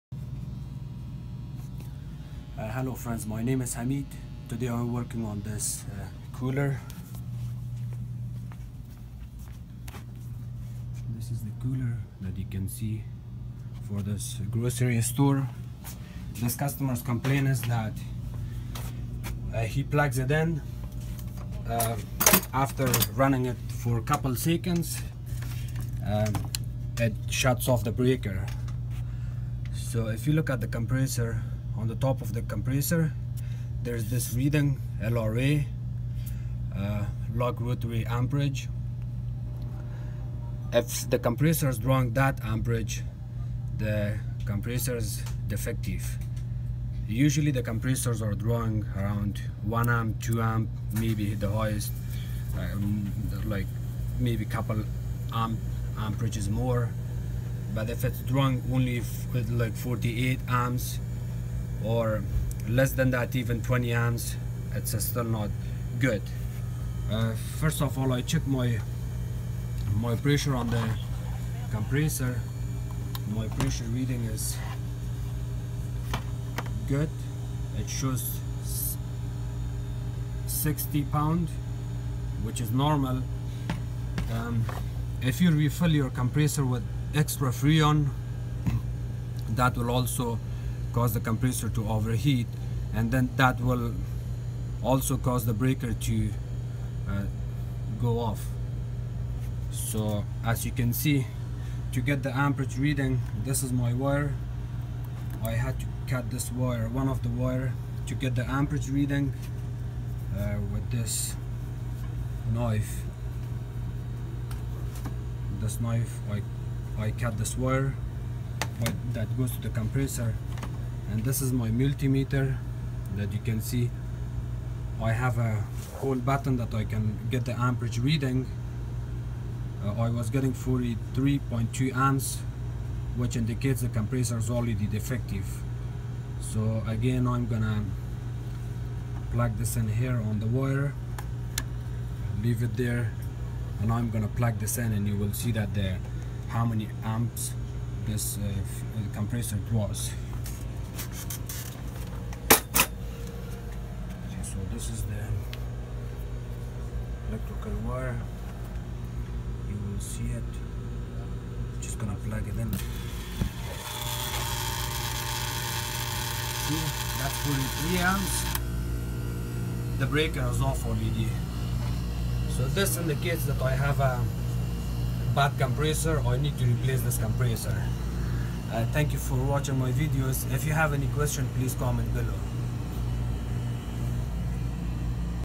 Fridge keeps tripping the circuit breaker
Had defective compressor,